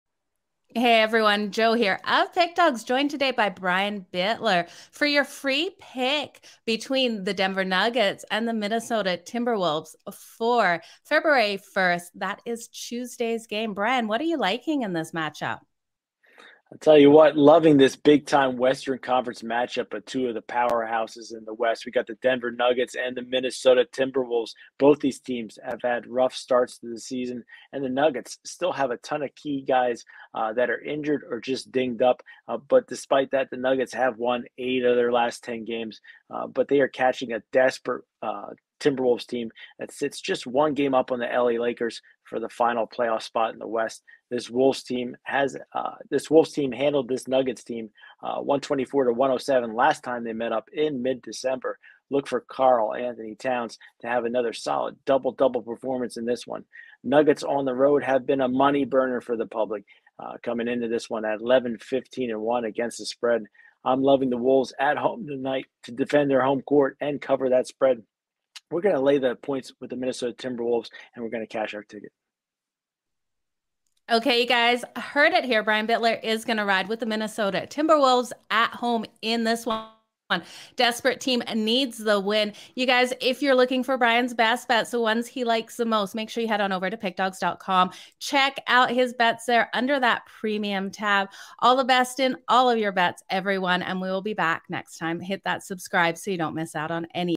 Minnesota Timberwolves vs Denver Nuggets 2/1/22 Free NBA Pick and Prediction NBA Betting Tips
The Denver Nuggets face the Minnesota Timberwolves in Tuesday's NBA Basketball action. Get Mitch's NBA Pick and Prediction for this game.